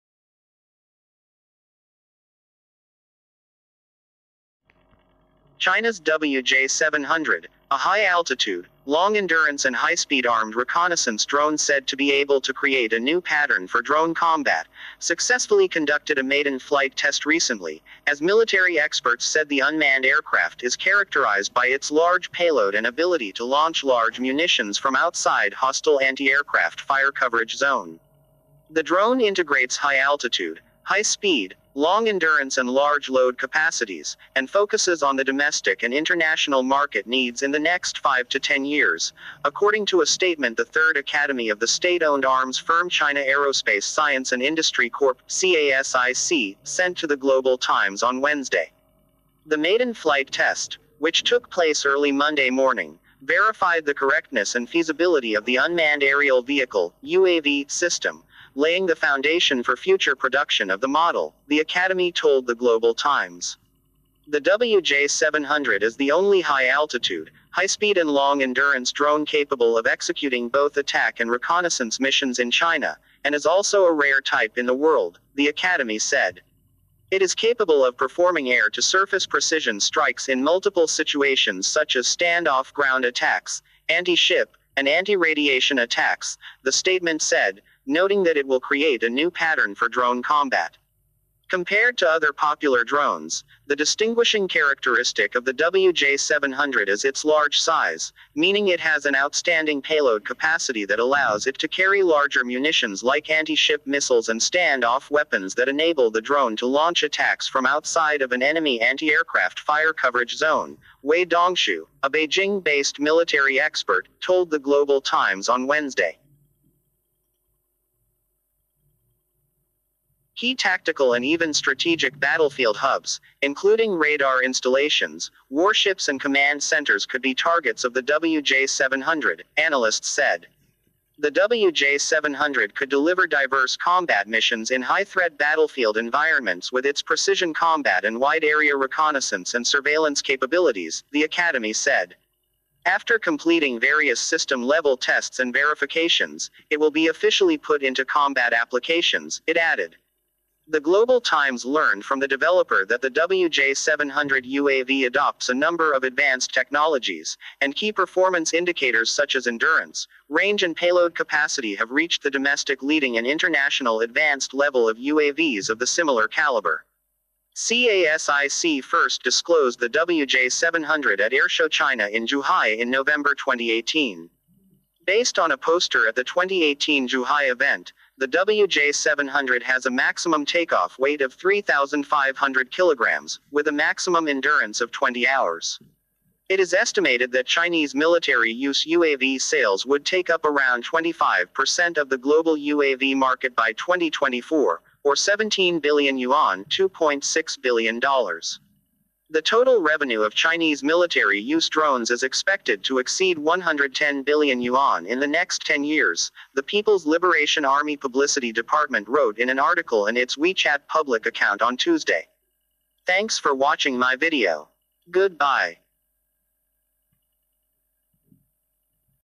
China’s WJ-700 drone completes maiden flight, ‘creates new drone combat pattern’
Key tactical and even strategic battlefield hubs, including radar installations, warships and command centers could be targets of the WJ-700, analysts said.

The WJ-700 could deliver diverse combat missions in high-threat battlefield environments with its precision combat and wide-area reconnaissance and surveillance capabilities, the academy said.

After completing various system-level tests and verifications, it will be officially put into combat applications, it added.

The Global Times learned from the developer that the WJ-700 UAV adopts a number of advanced technologies, and key performance indicators such as endurance, range and payload capacity have reached the domestic leading and international advanced level of UAVs of the similar caliber.

CASIC first disclosed the WJ-700 at Airshow China in Zhuhai in November 2018.

Based on a poster at the 2018 Zhuhai event, the WJ-700 has a maximum take-off weight of 3,500 kilograms, with a maximum endurance of 20 hours.

It is estimated that Chinese military-use UAV sales would take up around 25 percent of the global UAV market by 2024, or 17 billion yuan ($2.6 billion). The total revenue of Chinese military-use drones is expected to exceed 110 billion yuan in the next 10 years, the People's Liberation Army publicity department wrote in an article on its WeChat public account on Tuesday.